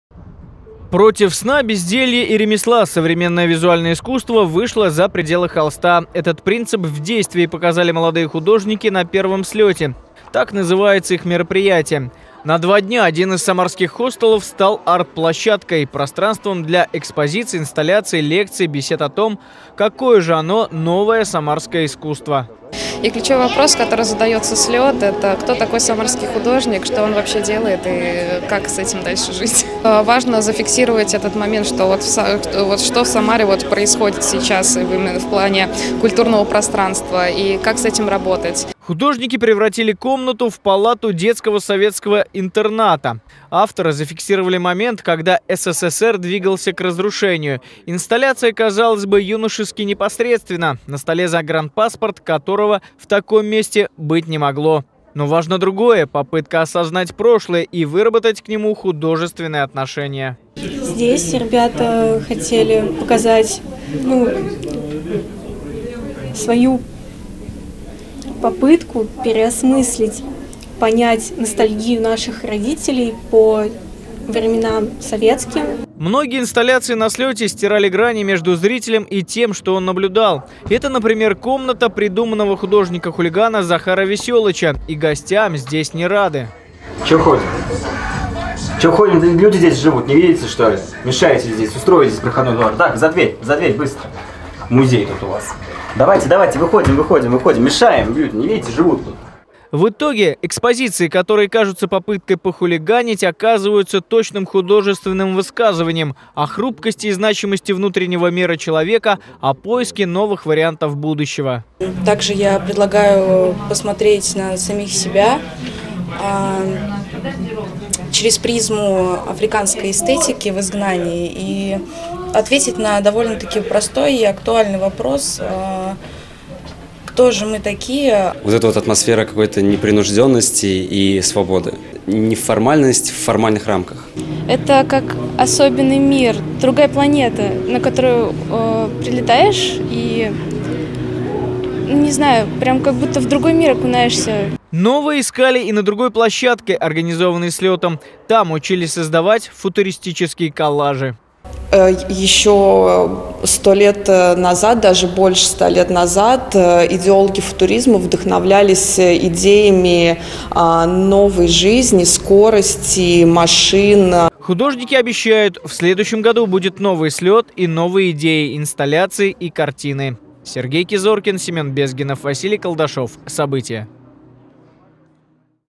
В арт-пространство превратился один из самарских хостелов
Против сна, безделья и ремесла.  .  Этот принцип в действии показали молодые художники на первом "Слете" - так называется их мероприятие. На два дня один из самарских хостелов стал арт-площадкой. пространством для экспозиций, инсталяций, лекций, бесед о том, какое же оно, новое самарское искусство.